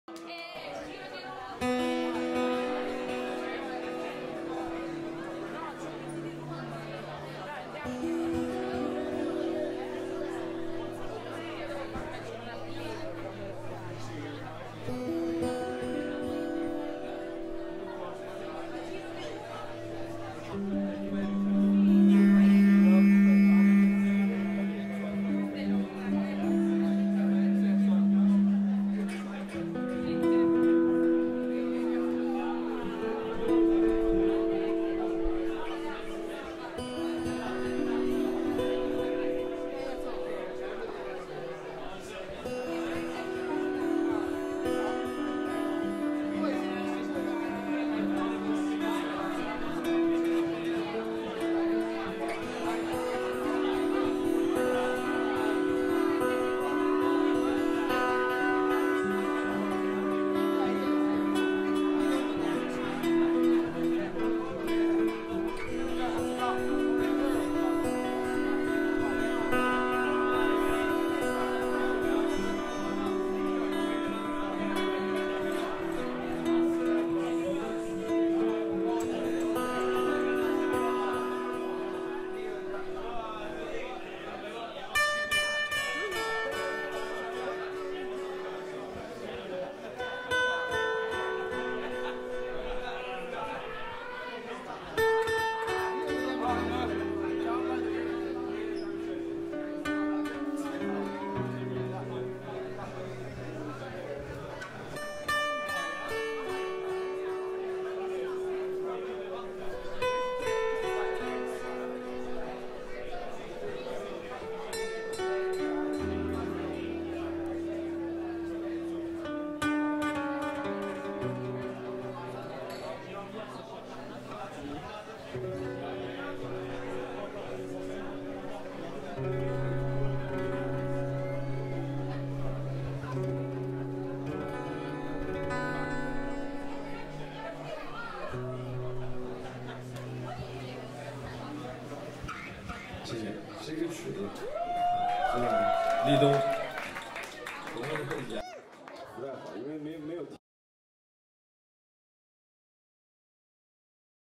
Li Dong 立東 at Dos Kolegas June 2012
Li Dong 立東 performing at Dos Kolegasl in May 2012. For more go to livebeijingmusic.com